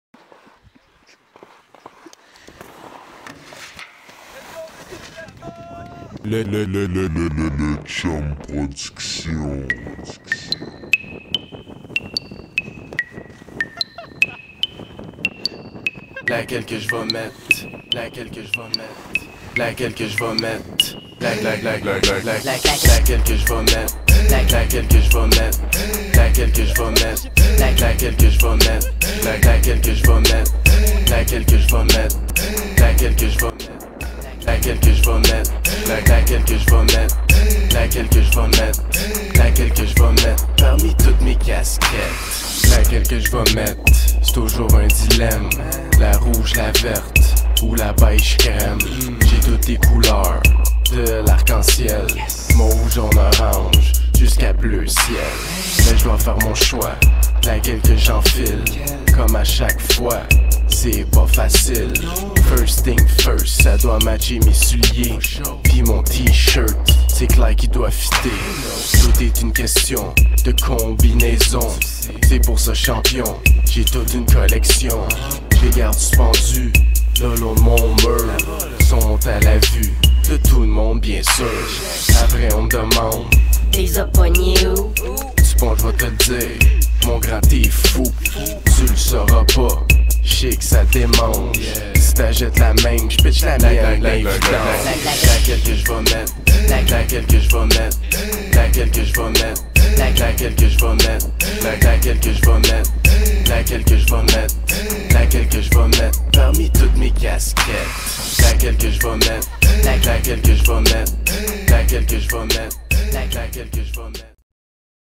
top to bottom Samuel Tardif
un autre vidéo pour le concours top to bottom cette fois avec le style afterbang de samuel tardif-joubert